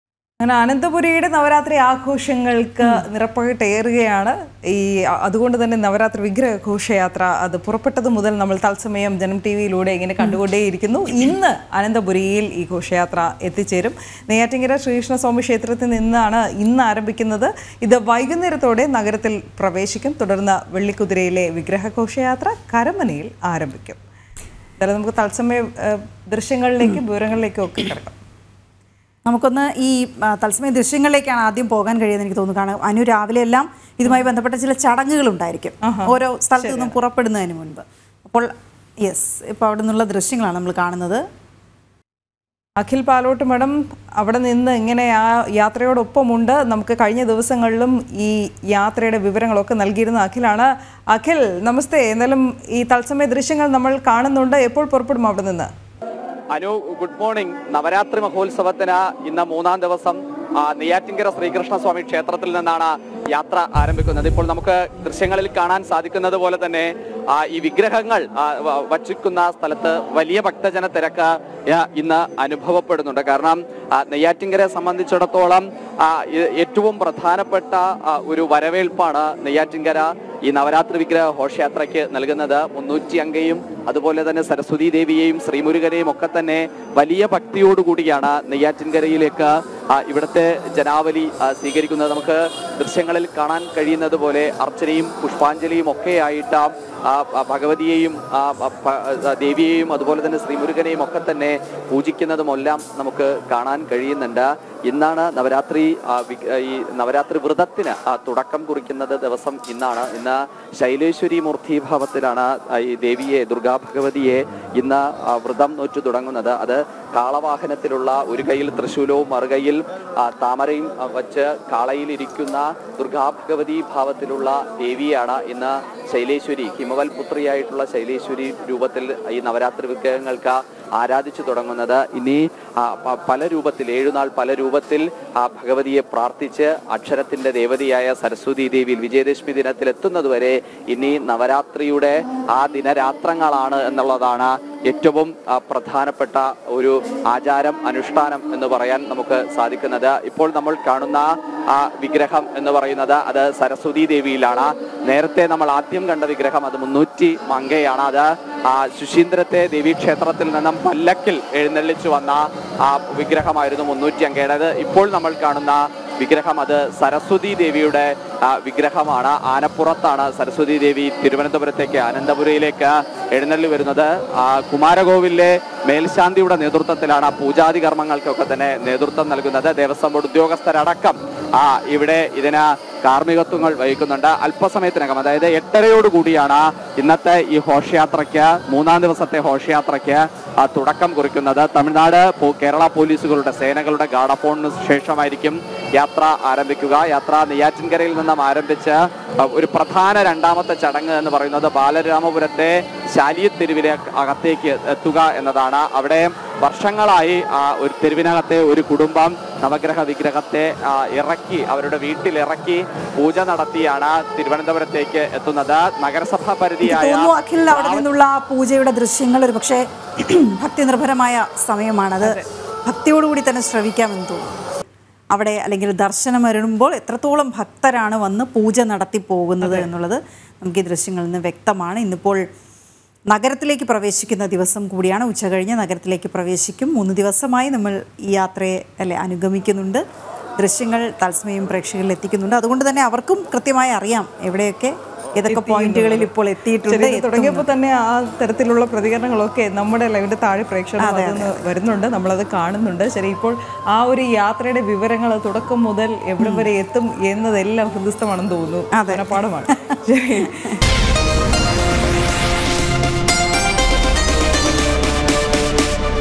നെയ്യാറ്റിൻകര ശ്രീകൃഷ്ണസ്വാമി ക്ഷേത്രത്തിൽ നിന്ന് ആരംഭിക്കുന്ന ഘോഷയാത്ര വൈകുന്നേരം നഗരത്തിലെത്തും
നവരാത്രി വിഗ്രഹ ഘോഷയാത്ര അനന്തപുരിയിലേക്ക്. നെയ്യാറ്റിൻകര ശ്രീകൃഷ്ണസ്വാമി ക്ഷേത്രത്തിൽ നിന്ന് അൽപസമയത്തിനകം ആരംഭിക്കുന്ന ഘോഷയാത്ര വൈകുന്നേരത്തോടെ നഗരത്തിൽ പ്രവേശിക്കും. തുടർന്ന് വെള്ളിക്കുതിരയിലെ വിഗ്രഹ ഘോഷയാത്ര കരമനയിൽ ആരംഭിക്കും.